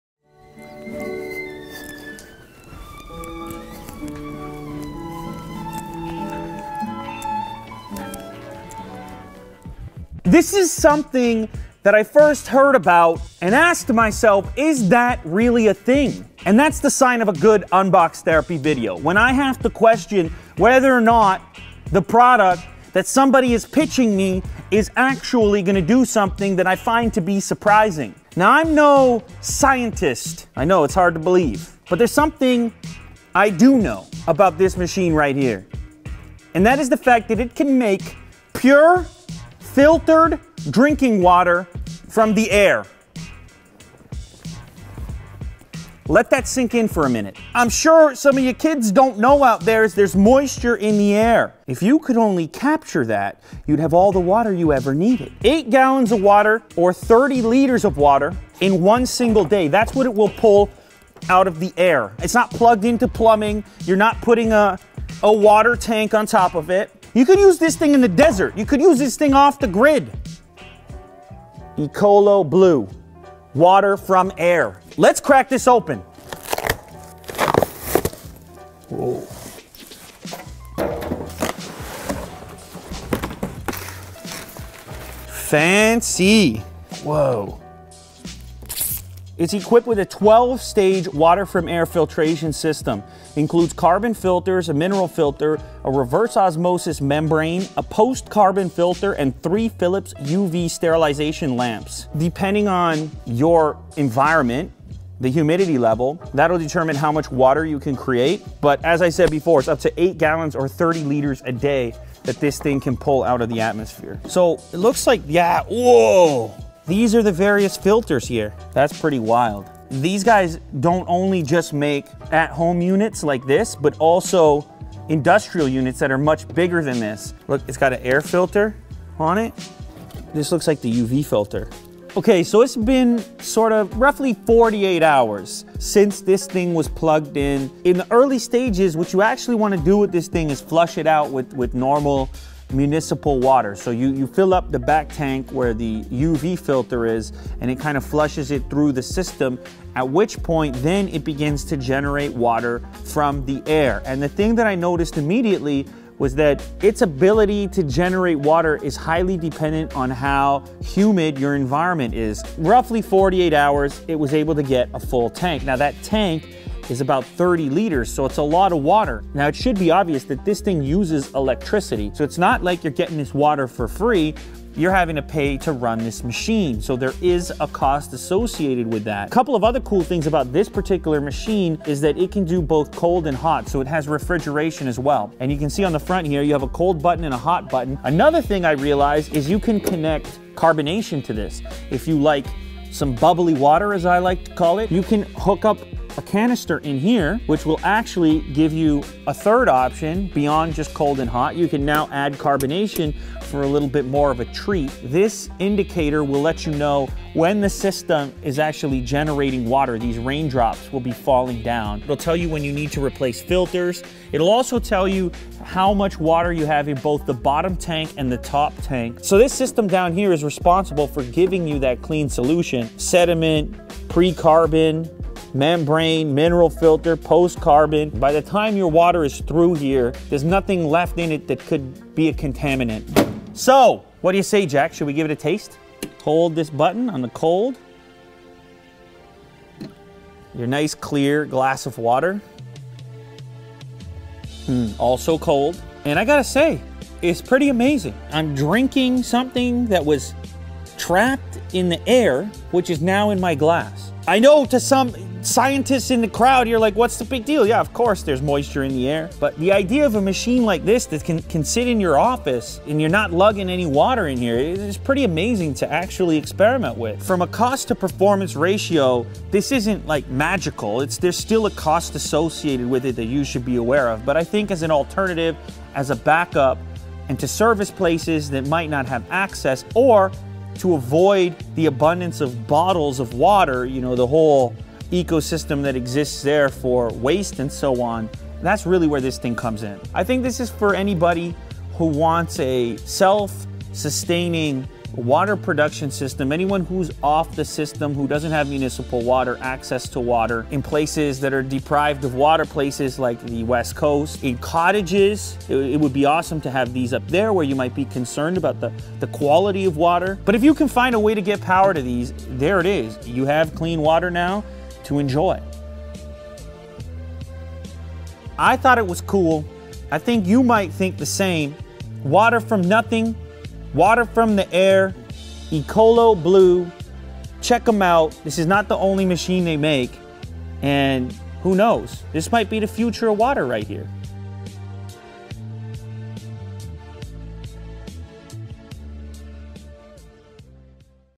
How To Turn Air Into Drinking Water
This machine from Ecoloblue takes moisture from the air and turns it into clean drinking water.

*This is a reupload from unbox therapy's channel, make sure too check him out.*

**Re-uploads for Unbox Therapy, please note that no videos are monetised .**